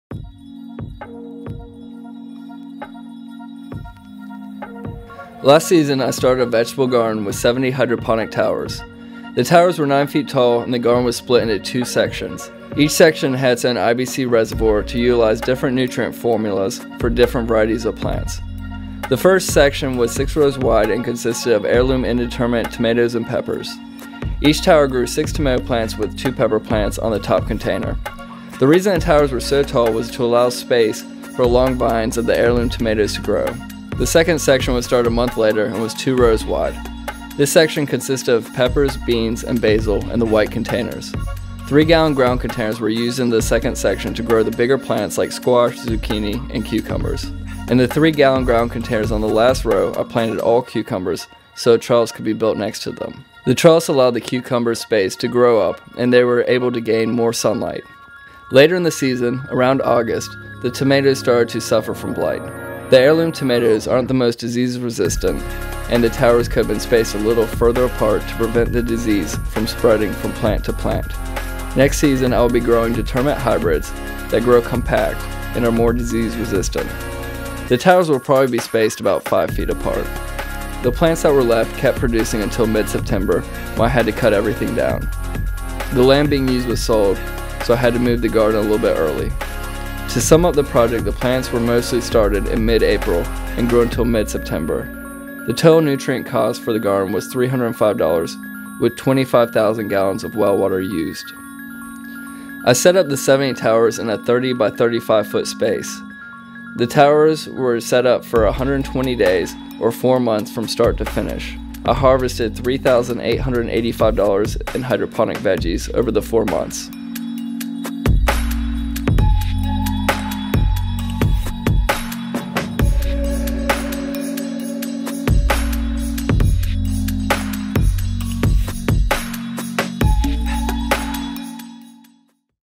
70 tower system - A season with vertical hydroponics
Growing summer varieties with a vertical hydroponic garden system. Recap on hydroponic veggies grown with water and nutrients used.